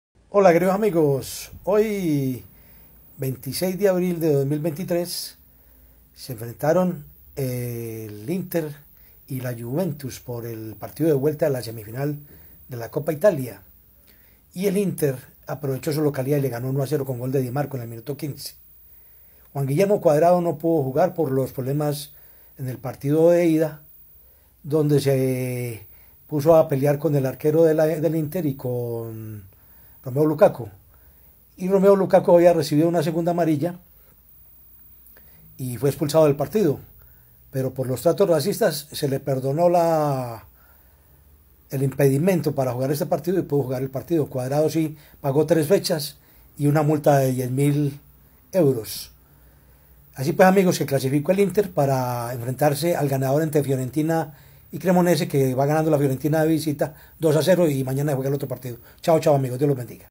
Inter de Milán a final Copa Italia
Éste miércoles 26 de abril de 2023, se jugó el partido de vuelta de la semifinal de la Copa Italia. El Inter en su estadio le ganó 1-0 a la Juventus, con gol de Dimarco al minuto 15. Luego de haber empatado en la ida 1-1. El Inter se clasificó a la final por la sumatoria de puntos...